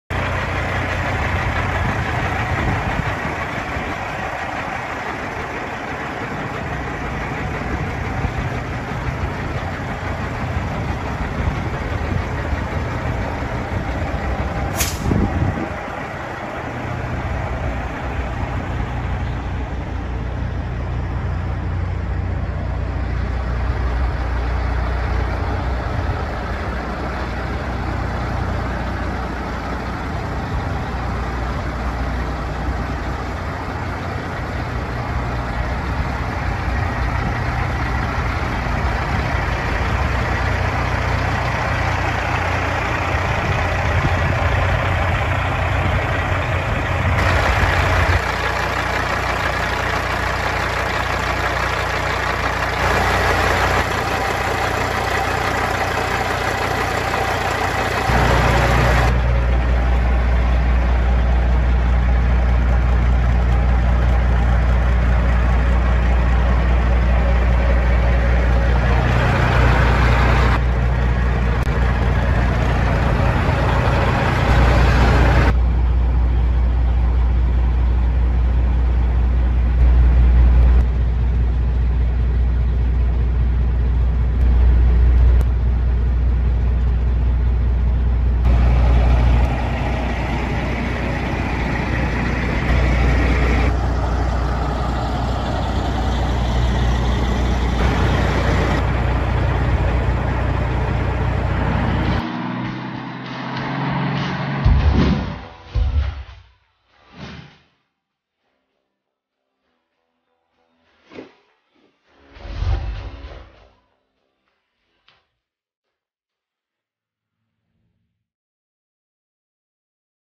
2005 KENWORTH W900L For Sale
2005 Kenworth W900L / CAT C15 545HP / 13 Speed / 86” Studio Sleeper

~ 3.55 Rears

~ 285” Wheelbase

~ 86” Studio Sleeper

~ VIT Interior

~ NEW 1/4 Fenders

Priced at $